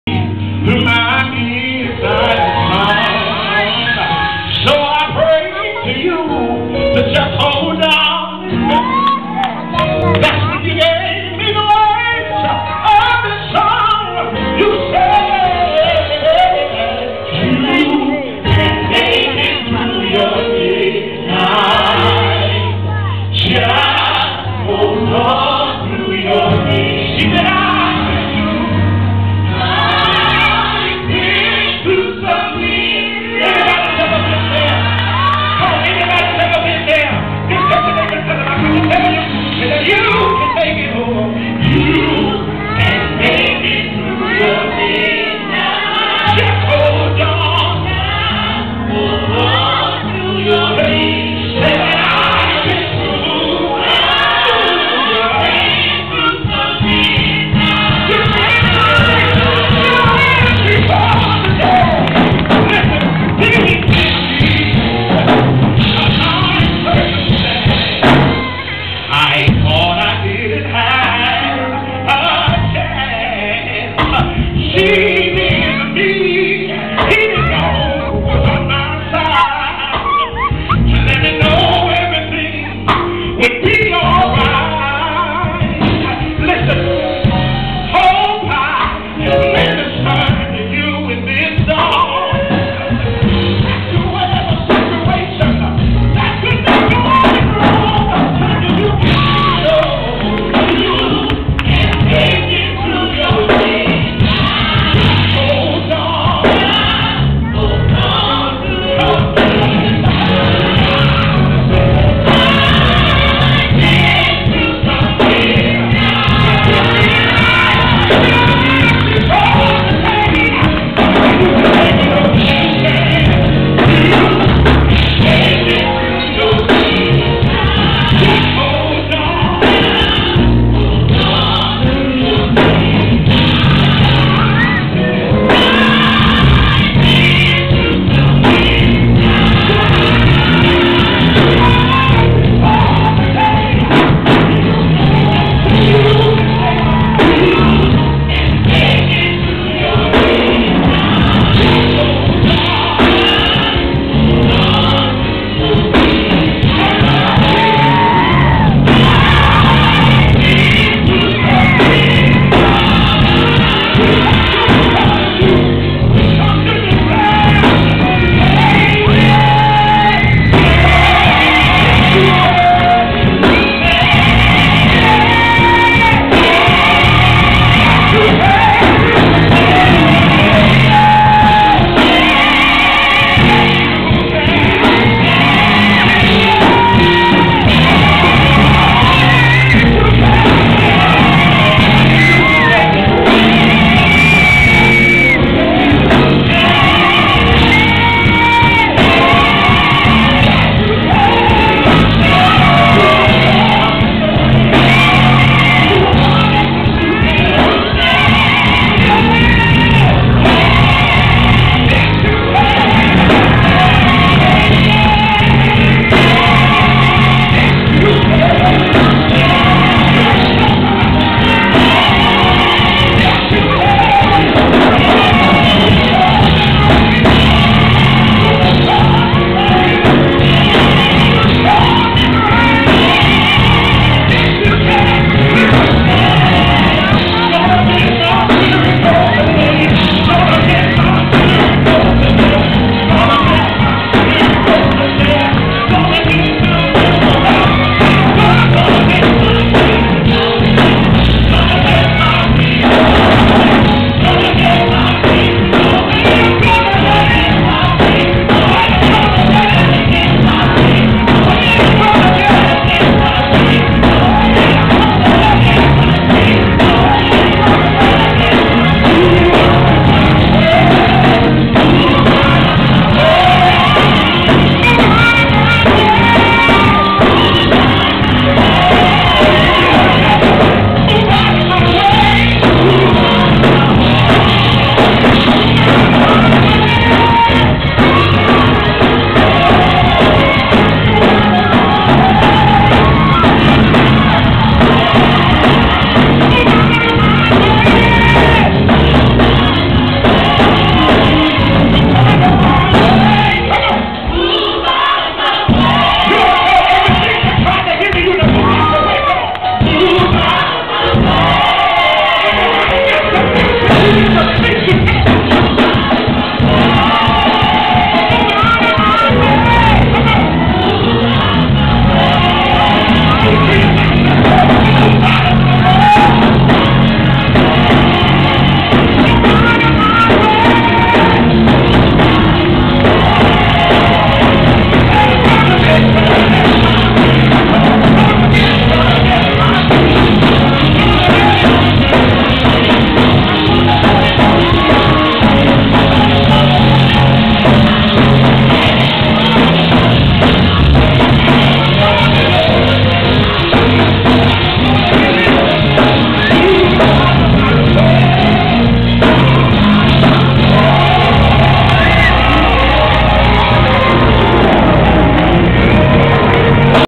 Marquise Belchor & Devine Purpose
Marquise Belchor and DP in Eastman Ga at the Sunset Family Worship Center.. 2009 Gospel bash